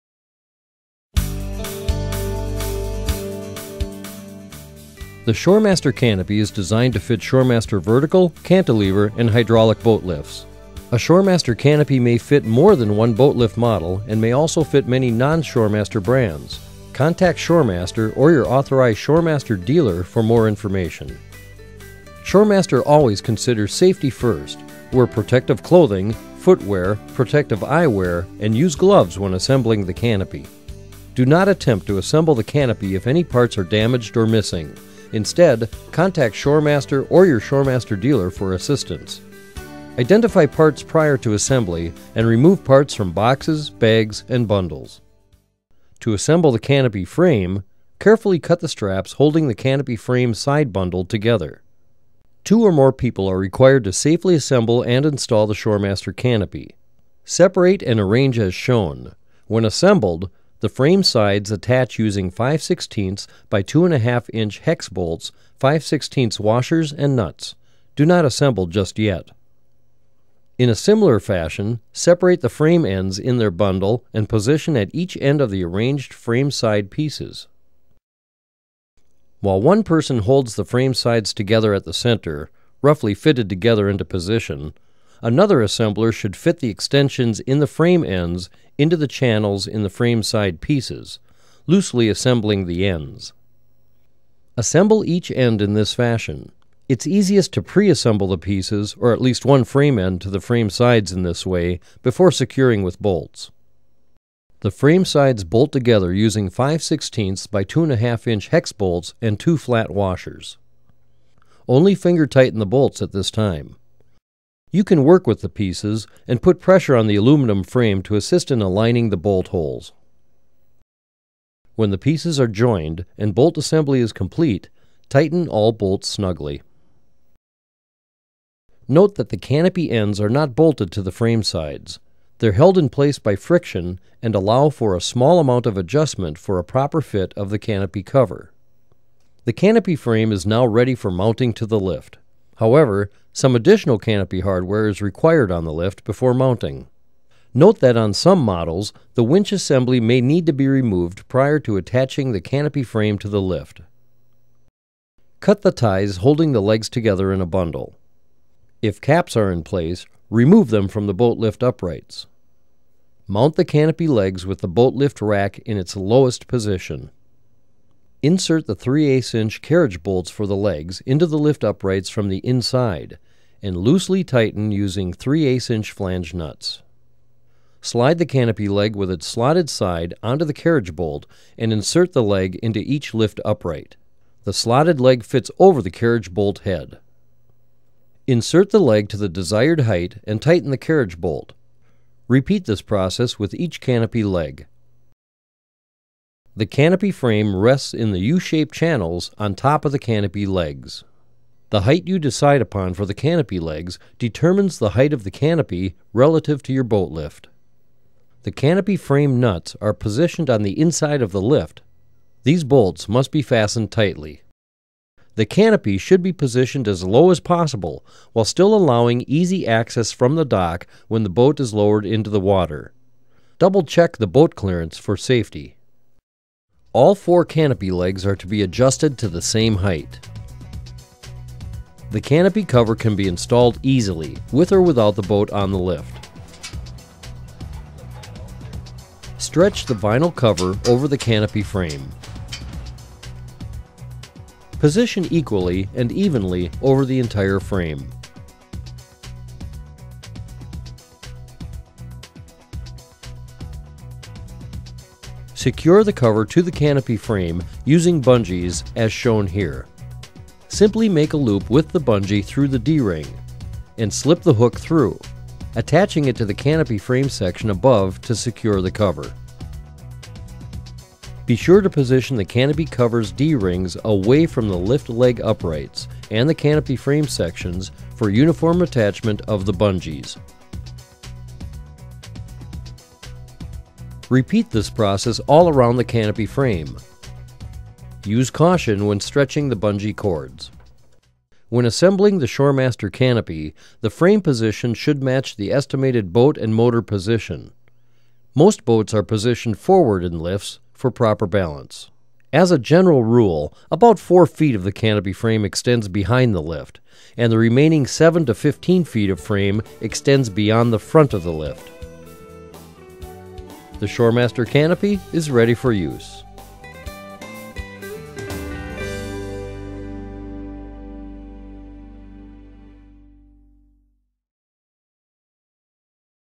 ShoreMaster Canopy Frame and Vinyl
This video shows you how to assemble, and install a ShoreMaster Boat Lift Canopy Frame and vinyl cover. Use the dealer locator at shoremaster.com to find a canopy for your boat!